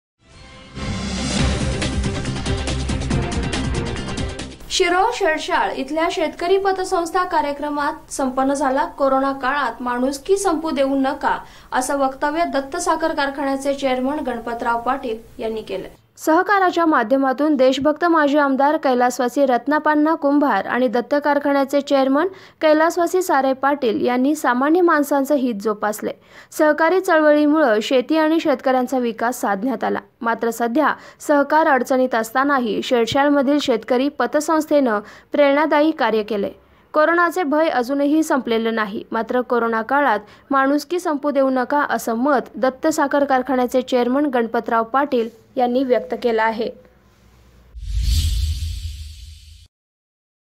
Kolhapur | कोरोना काळात माणुसकी संपू देवू नका - दत्त साखर कारखान्याचे चेअरमन गणपतराव पाटील
कोरोना काळात माणुसकी संपू देवू नका - दत्त साखर कारखान्याचे चेअरमन गणपतराव पाटील.@